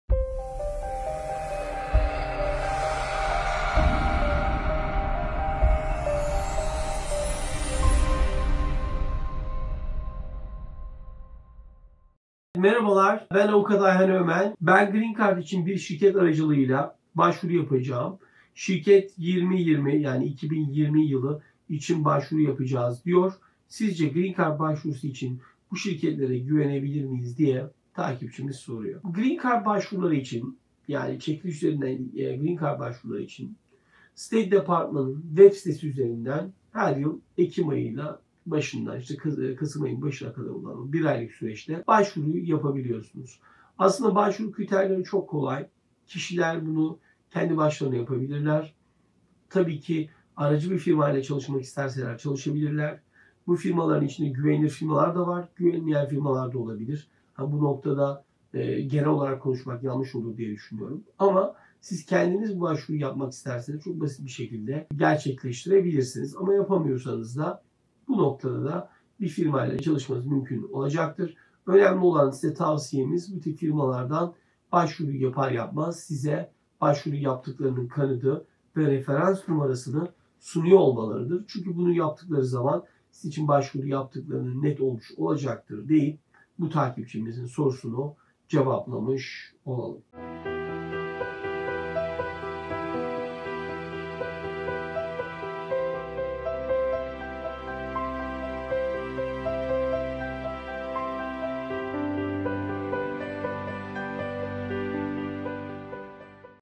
GREEN CARD ÇEKİLİŞ BAŞVURUSUNU ARACI FİRMALARLA YAPMAK GÜVENLİ MİDİR ?
Soru: Ben Green Card için bir şirket aracılığıyla başvuru yapacağım. Şirket 2020 yılı için başvuru yapacağız diyor. Sizce Green Card başvurusu için bu şirketlere güvenebilir miyiz ?